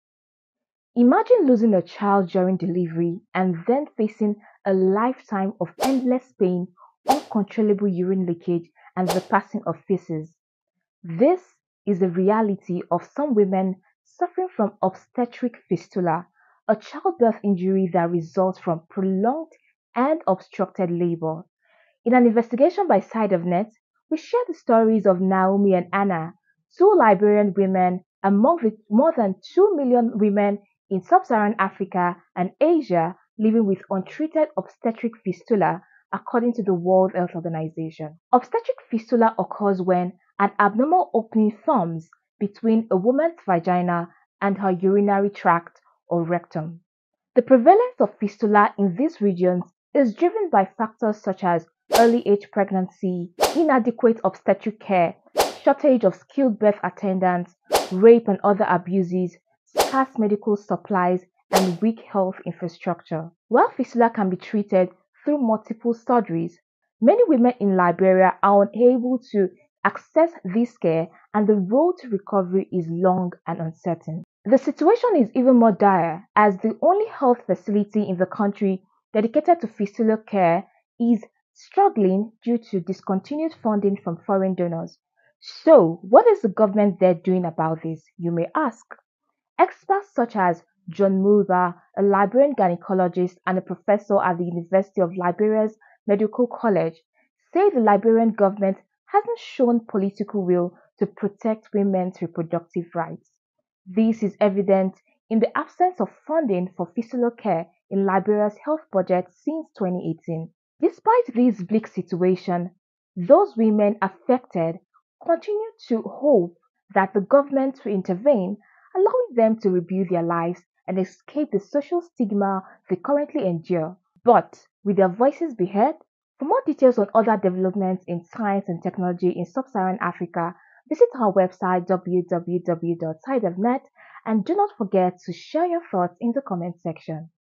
Fistula scars: Liberia’s forgotten women
Obstetric Fistula, a preventable childbirth injury, leaves Liberian women incontinent, isolated. The country’s only fistula care centre hit by loss of funding. Also,  lack of data on fistula cases hinders effective response in the country